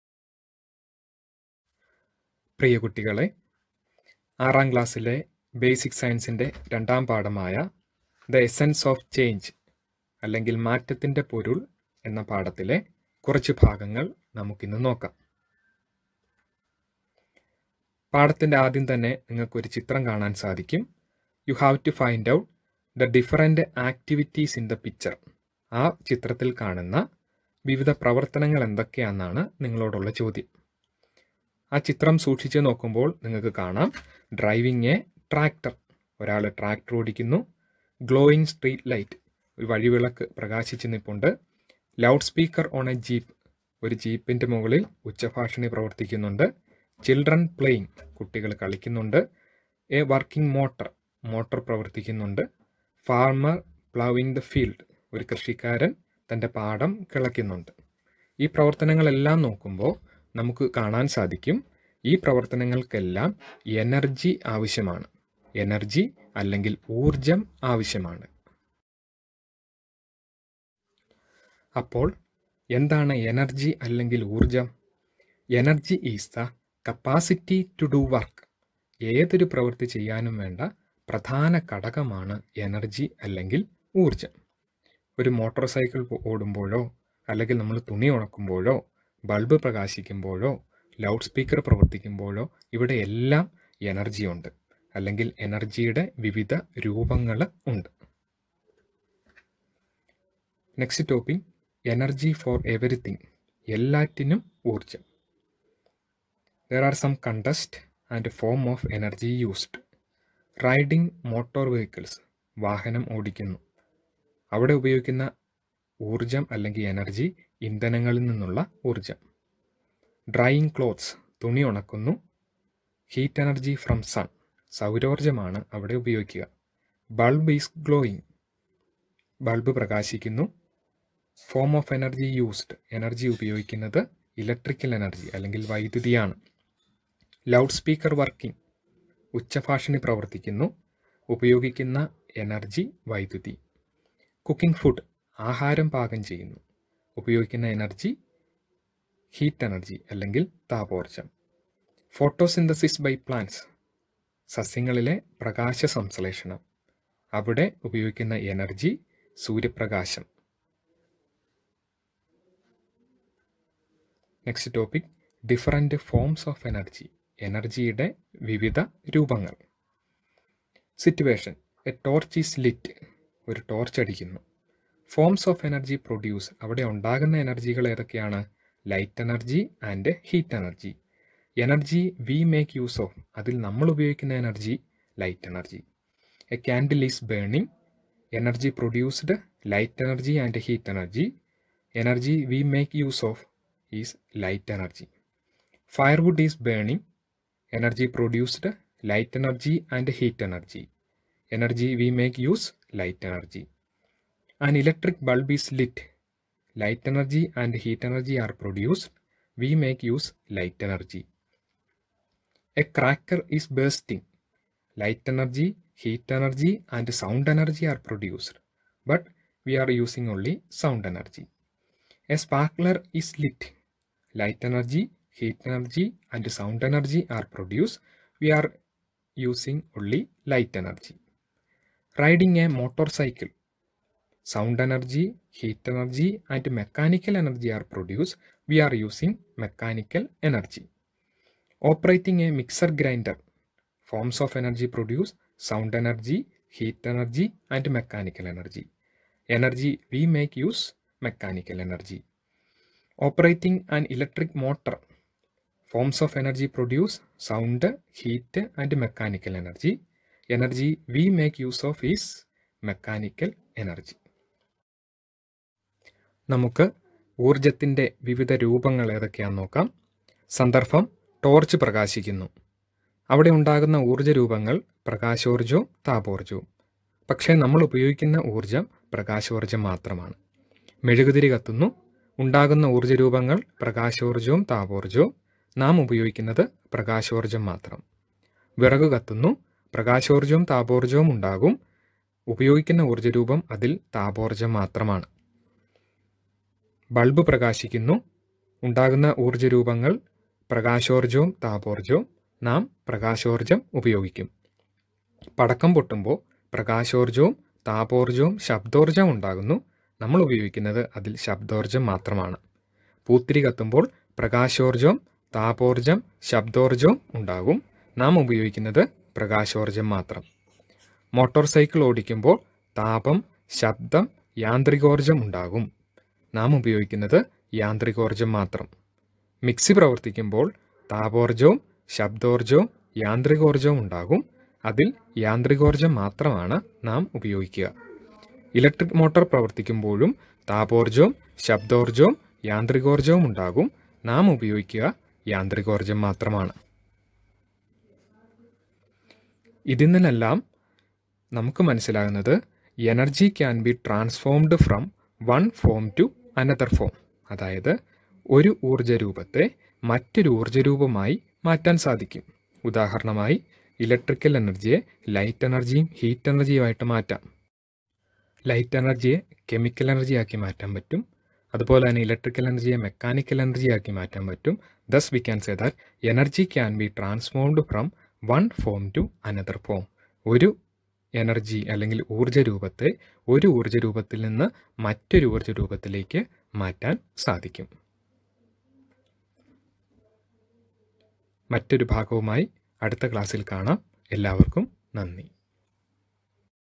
class 6|basic science|chapter 2|PART-1|the essence of change|മാറ്റത്തിന്റെപൊരുൾ |AV Tech Edu
video explains the standard 6 basic science chapter-2 the essence of change in detail.
It includes all the text book activities.

മാറ്റത്തിന്റെ പൊരുൾ പാഠഭാഗത്തിന്റെ ആദ്യഭാഗം